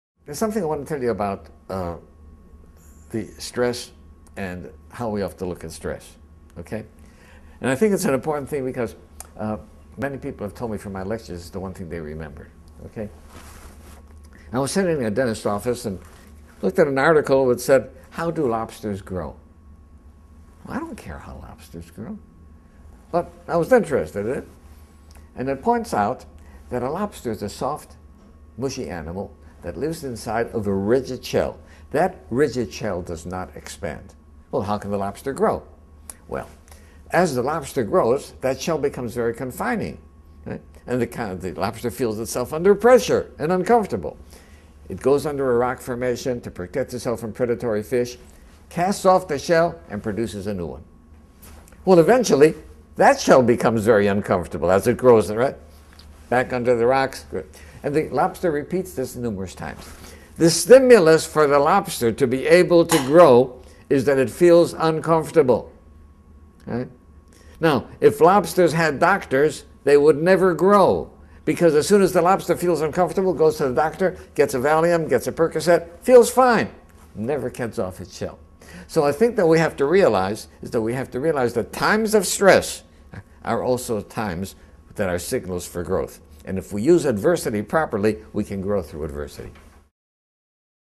OT 32.2 Lobsters: A view on stress and adversity
A short video where Rabbi Dr. Abraham Twerski speaks about responding to stress.